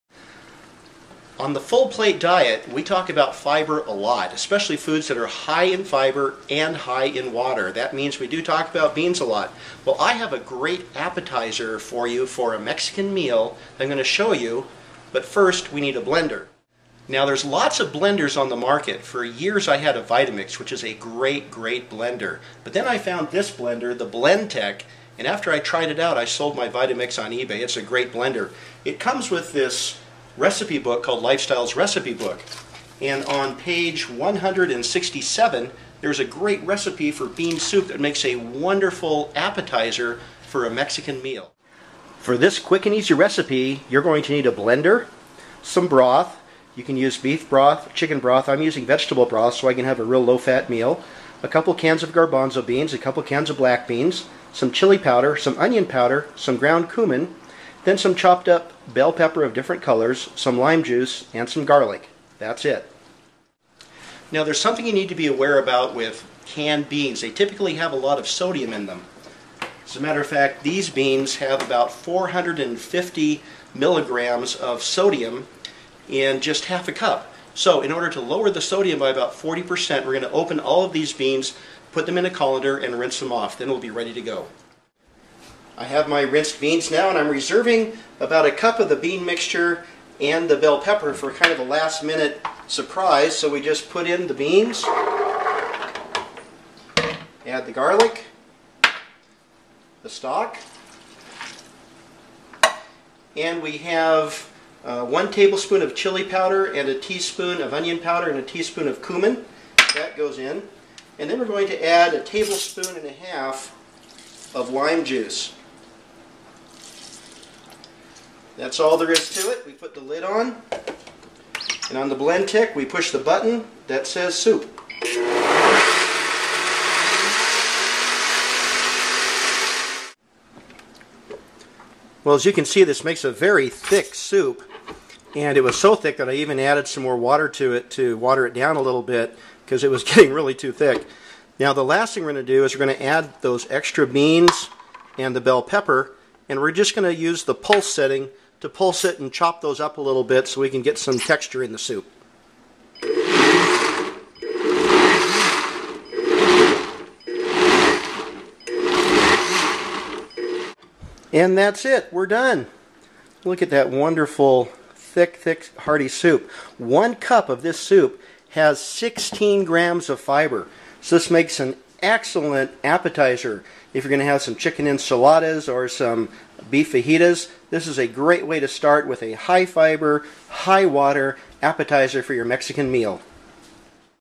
Mexican Bean Appetizer Soup from the Full Plate Diet
Need a great high-fiber soup as an appetizer for your chicken kabobs, beef fajitas, or tamales?  This quick and easy soup is ready in less than 7 minutes using a blender.  15 grams of fiber in a cup!